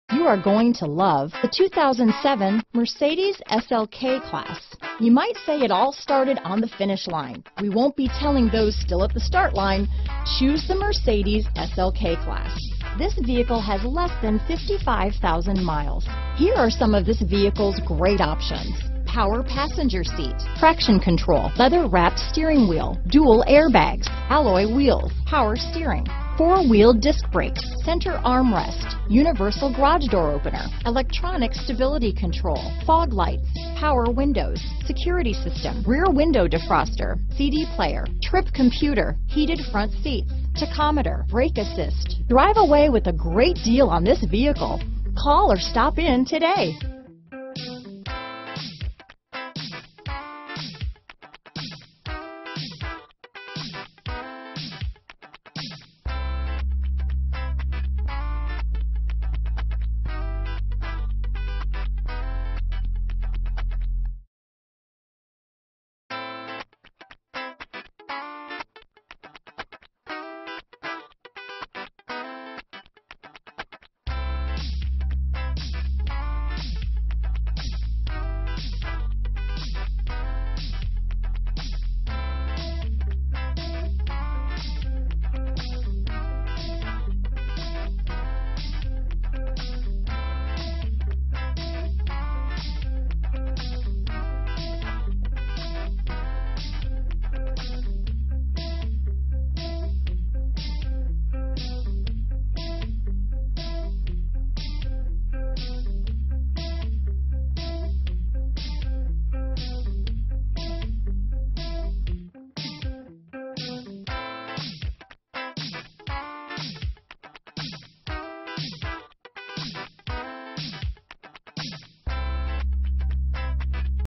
2007 Mercedes-Benz SLK Schaumburg, Arlington Heights, Hoffman Estates, Des Plaines, Palatine, IL 161
Black Used 2007 Mercedes-Benz SLK available in Schaumburg, Illinois at Schaumburg Honda. Servicing the Arlington Heights, Hoffman Estates, Des Plaines, Palatine, IL area.

Schaumburg Honda
750 E Golf Rd

Recent Arrival! 2007 Mercedes-Benz RWD SLK SLK 55 AMG Black 7-Speed Automatic Clean CARFAX.